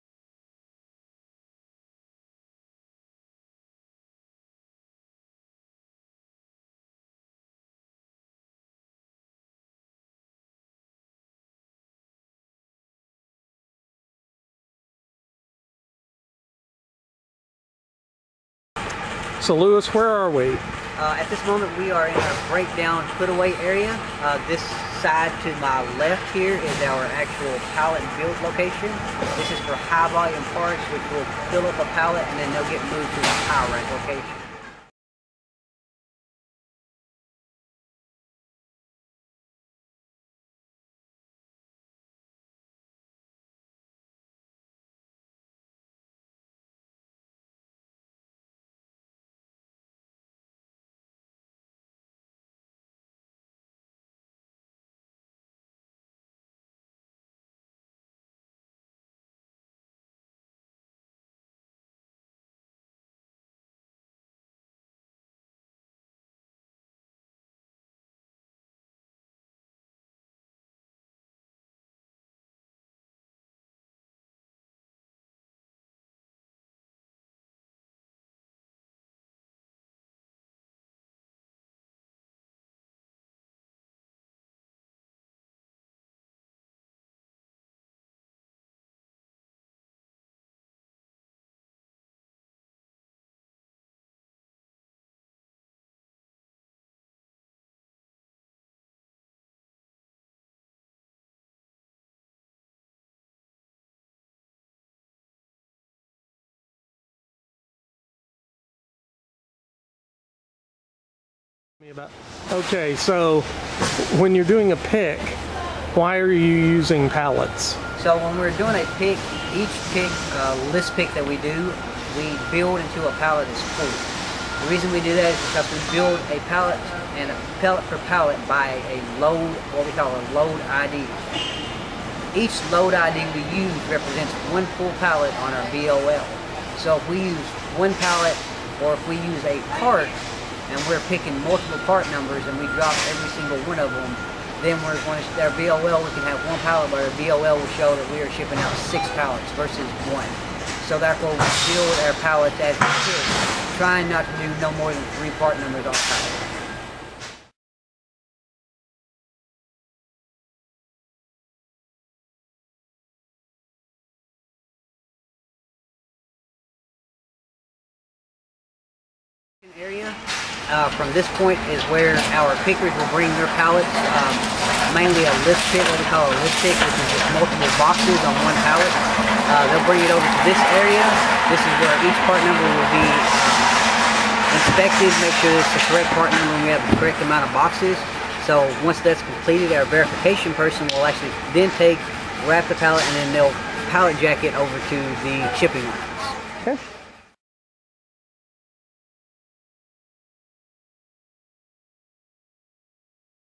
Warehouse Operation
Order breakdown, putaway and picking up to assembled pallet verification and movement to shipping dock.